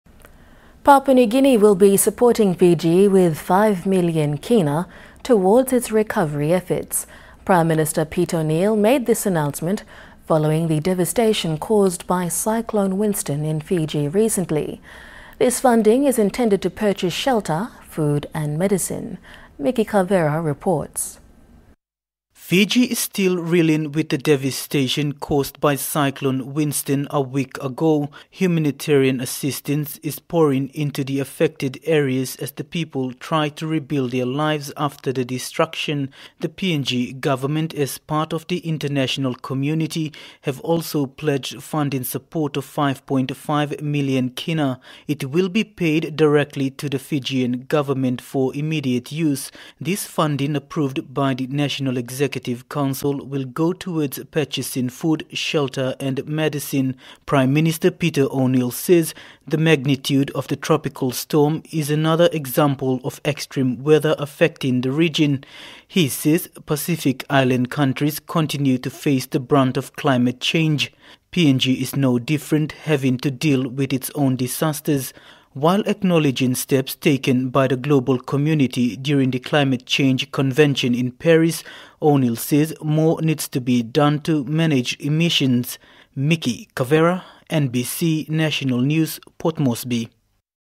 PNG Assist Fiji_NBC News
Papua New Guinea will be supporting Fiji with K5 million towards its recovery efforts.